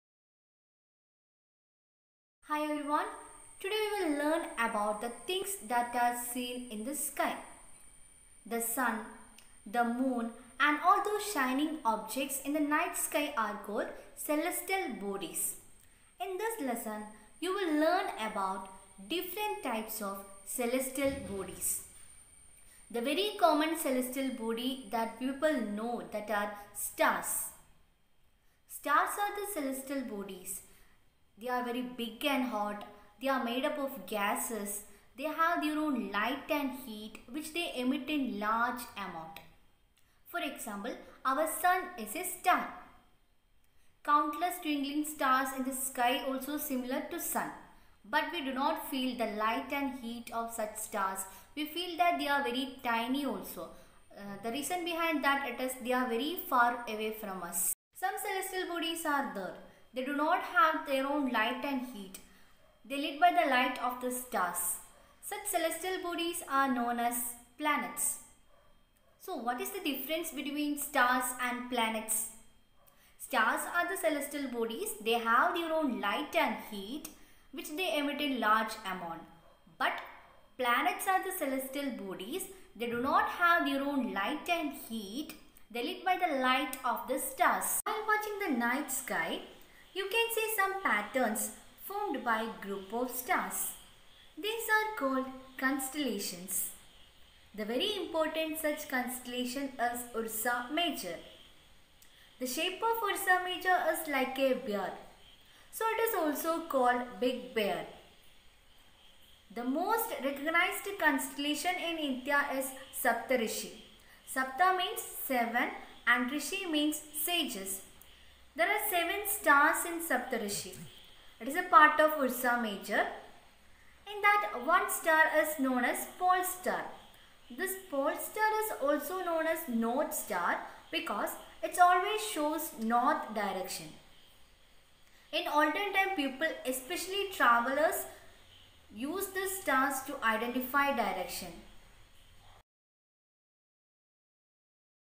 What are celestial bodies?, Difference between stars and planets, What are Constellations?
Video lessons for CBSE, Kerala State school students and other competitive examinations